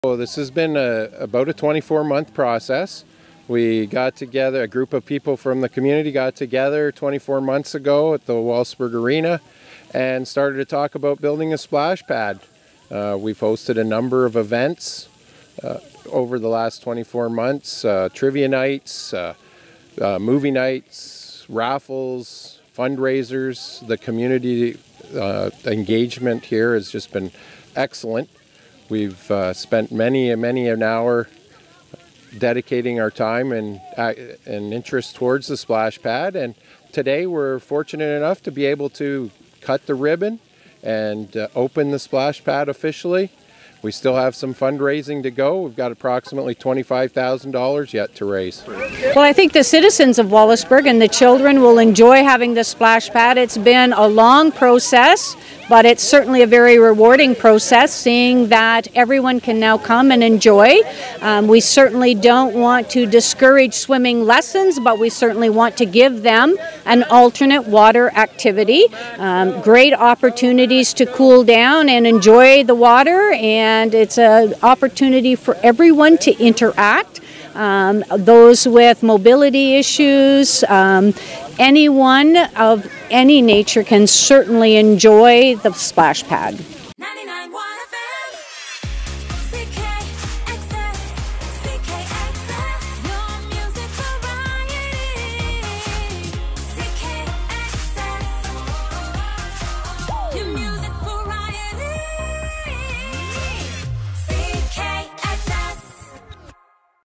Wallaceburg Splash Pad Ribbon Cutting Celebration
The ribbon cutting celebrations took place on Saturday to celebrate the completion of the Wallaceburg Splash Pad.